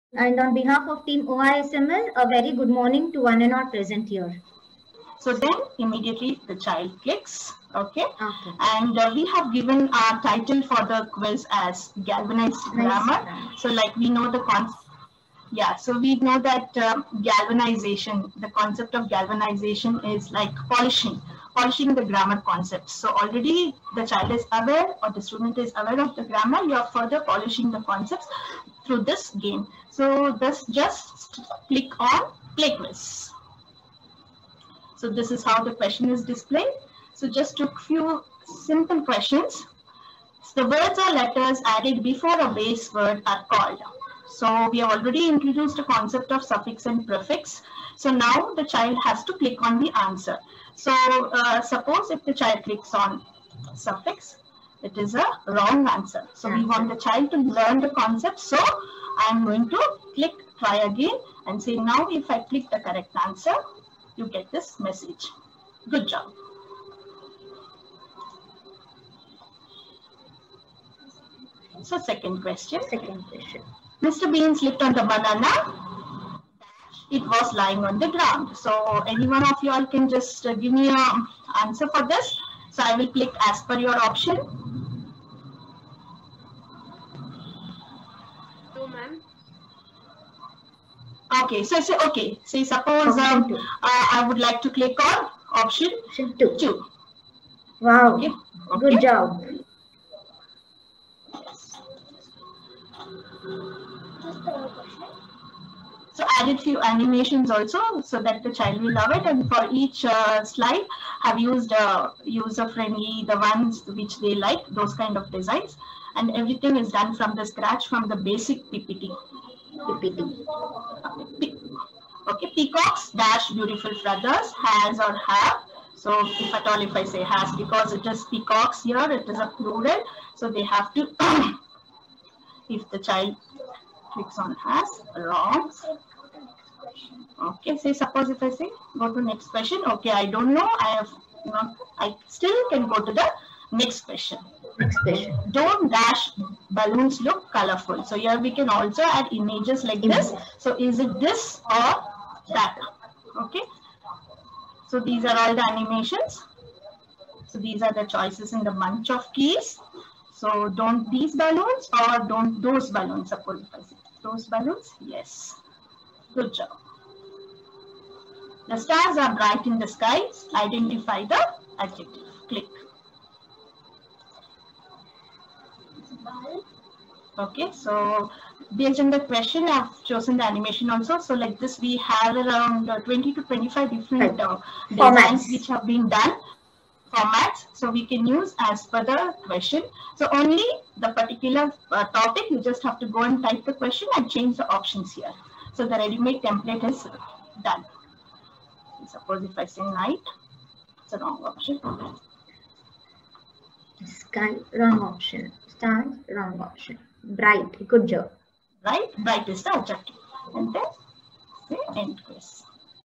Educational Game for Teaching English and EVS by Ms. Hemalata and Ms. Ashwini
FAME-A-GAME, the National level- Inter-School competition for all teachers. It is an educational game competition

This game is about learning how to galvanised the grammar of your students

Presented By : Ms. Hemalata and Ms.Ashwini
Orchids The International School, Mahalakshmi